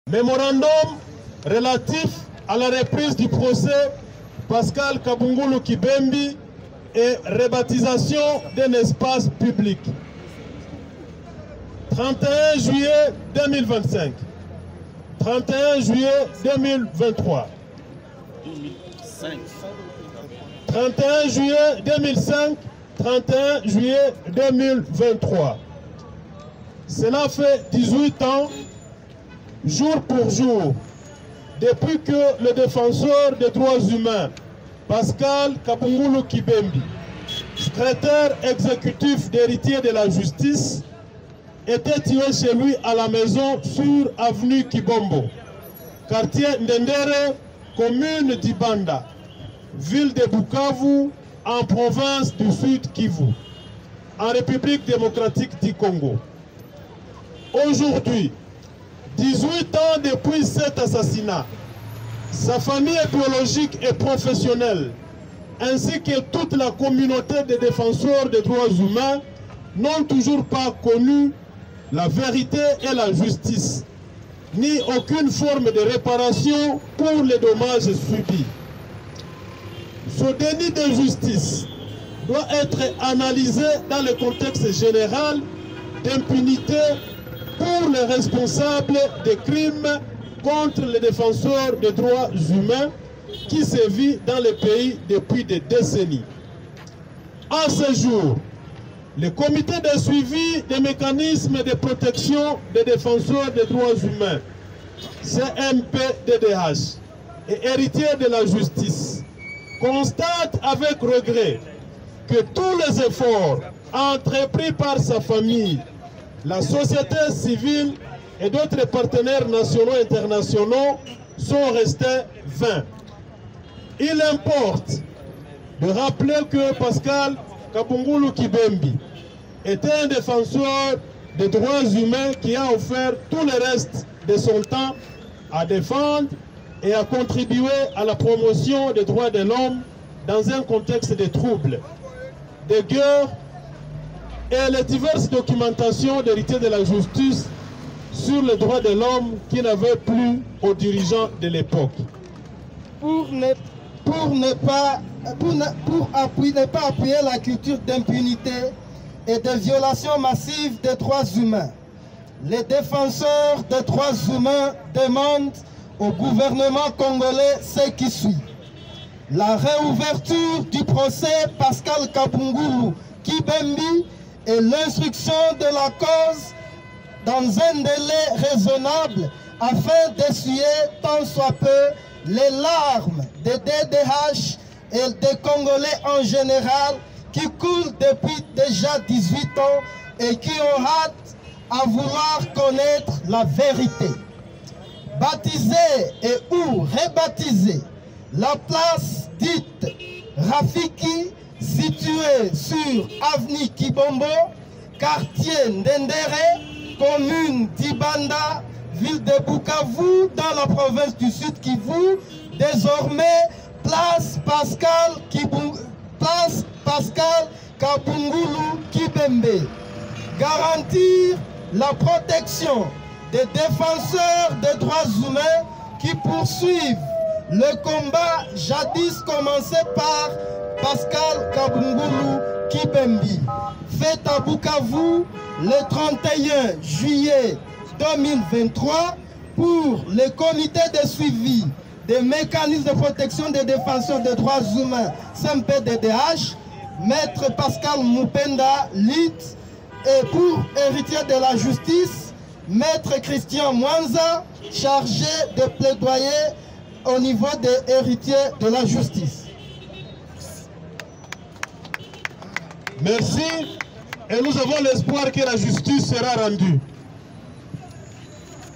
18 ans après, Kabungulu mérite d'être immortalisé, justice doit être faite!
Les organisations de défense des droits de l’homme réunies au sein du Comité de Suivi des mécanismes de protection des défenseurs des Droits Humains réclament la réouverture du procès Pascal Kabungulu. Ce lundi 31 juillet 2023, 18 ans après l’assassinat de Pascal Kabungulu Kibembi, les défenseurs des droits de l’homme ont organisé à Bukavu plusieurs activités en mémoire de ce vaillant défenseur des droits de l’homme, assassiné à son domicile à Bukavu en 2005.